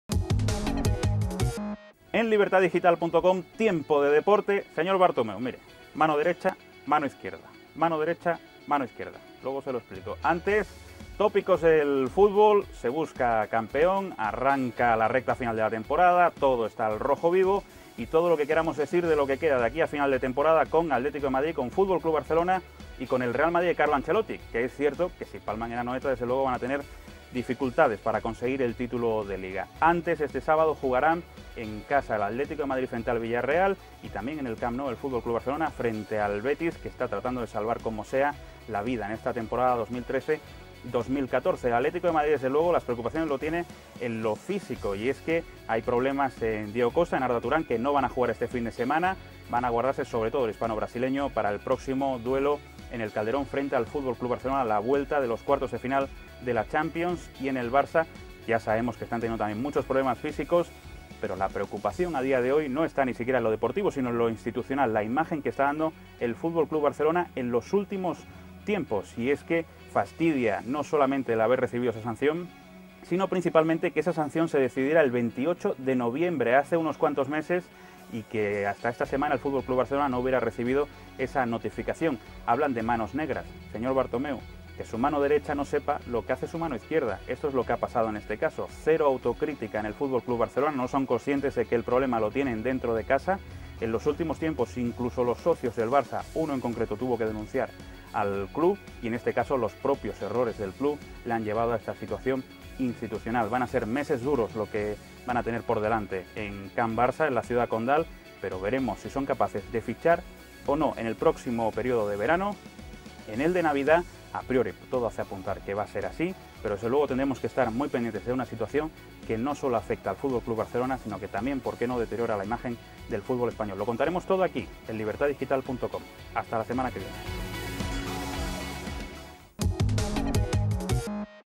La "mano negra" que mece la cuna del Barça
En Libertaddigital.com, tiempo de deporte. Señor Bartomeu, mire. Mano derecha, mano izquierda, mano derecha, mano izquierda. Luego se lo explico. Antes, tópicos del fútbol. Se busca campeón, arranca la recta final de la temporada, todo está al rojo vivo y todo lo que queramos decir de lo que queda de aquí a final de temporada con Atlético de Madrid, con Fútbol Club Barcelona y con el Real Madrid de Carlo Ancelotti, que es cierto que si palma mañana en Anoeta va a tener dificultades para conseguir el título de Liga.

Antes, este sábado, jugará en casa el Atlético de Madrid frente al Villarreal y también en el Camp Nou el Fútbol Club Barcelona frente al Betis que está tratando de salvar como sea la vida esta temporada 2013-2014. En el Atlético de madrid, desde luego, la preocupación la tiene en lo físico. Y es que hay problemas en Diego Costa y Arda Turán que no van a jugar este fin de semana. Van a guardarse, sobre todo el hispano-brasileño, para el próximo duelo en el Calderón frente al Fútbol Club Barcelona la vuelta de los cuartos de final de la Champions.

Y en el Barça, ya sabemos que están teniendo también muchos problemas físicos, pero la preocupación a día de hoy no está ni siquiera en lo deportivo sino en lo institucional. La imagen que está dando el Fútbol Club Barcelona en los últimos tiempos. Y es que fastidia no solamente el haber recibido esa sanción si no principalmente que esa sanción se decidiera el 28 de noviembre, hace cuatro meses, y que hasta esta semana el Fútbol Club Barcelona no hubiera recibido esa notificación.

Hablan de manos negras. Señor Bartomeu, que su mano derecha no sepa lo que hace su mano izquierda. Esto es lo que ha pasado en este caso. Cero autocrítica en el Fútbol Club Barcelona. No son conscientes de que el problema lo tienen dentro de casa. En los últimos tiempos incluso los socios del Barça, uno en concreto, tuvo que denunciar al club. Y en este caso los propios errores del club lo han llevado a esta situación institucional.

Van a ser meses duros los que van a tener por delante en Can Barça, en la ciudad condal, pero veremos si son capaces de fichar o no en el próximo período de verano y en el de Navidad. A priori, todo hace apuntar que va a ser así pero desde luego tendremos que estar que estar muy pendientes de una situación que no sólo afecta al Fútbol Club Barcelona sino que también, por qué no, deteriora la imagen del fútbol español. Lo contaremos todo aquí, en libertaddigital.com. Hasta la semana que viene.